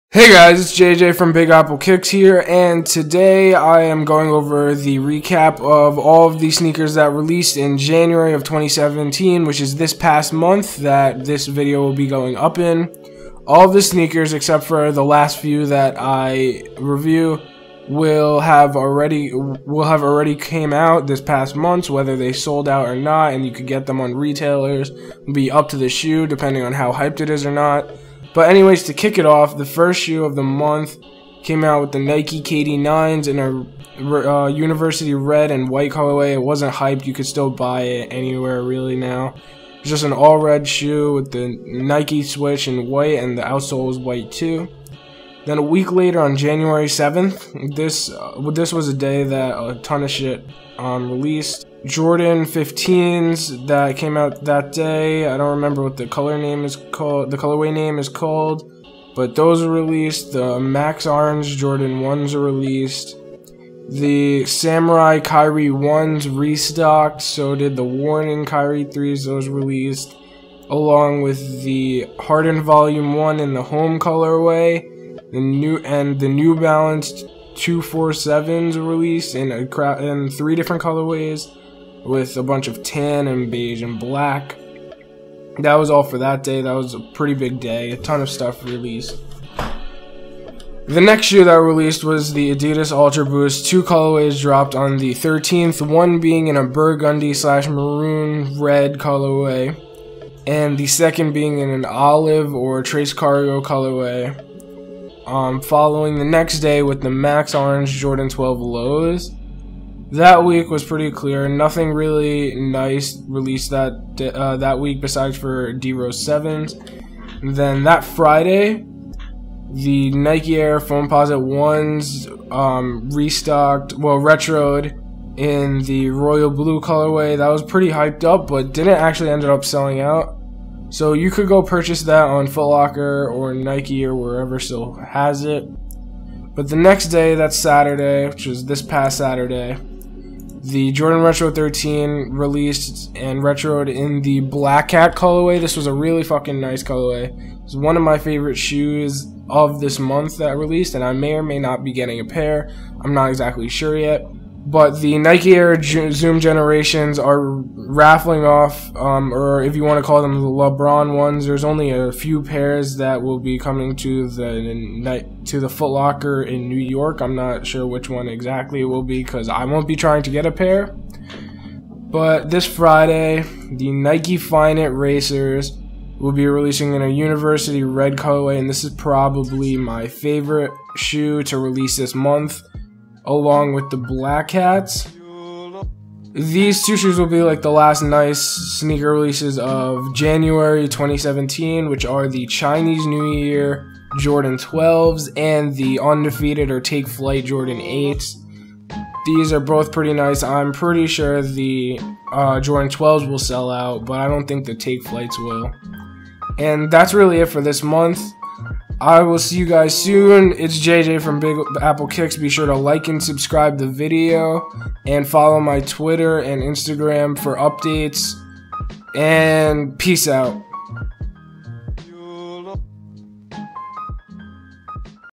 JANUARY 2017 SNEAKER RELEASE RECAP!!
Going over all of the sneaker releases of January 2017.